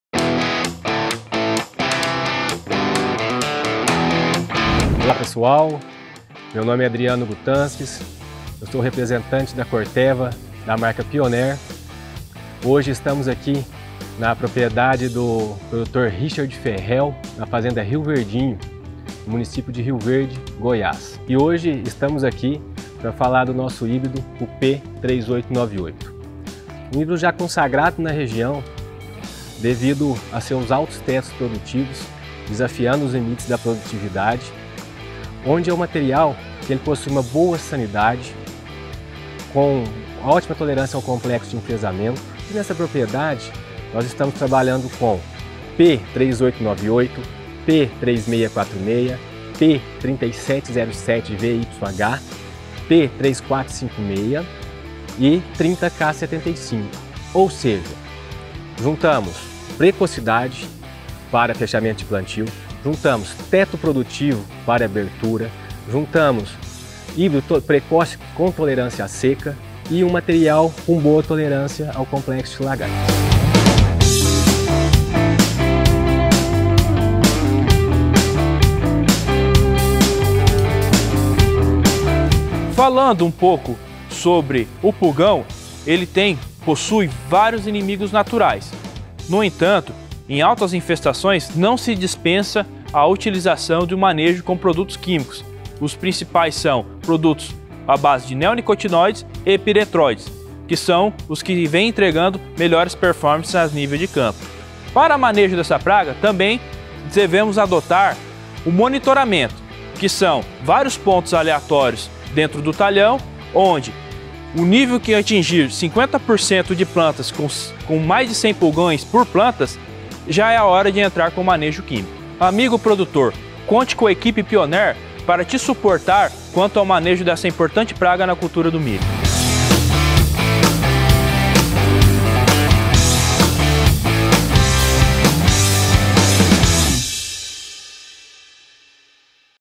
Dia do Campo Virtual - Portfólio de Milho no Centro - Fazenda Verdinha
Apresentamos o Dia de Campo Virtual para a região Centro, com um portfólio de híbridos para safrinha da marca Pioneer®️ e com recomendações técnicas para garantir sucesso na sua produtividade.

Dê play e conheça os nossos Grandes Produtos!

Consulte um representante autorizado da Pioneer®️ na sua região.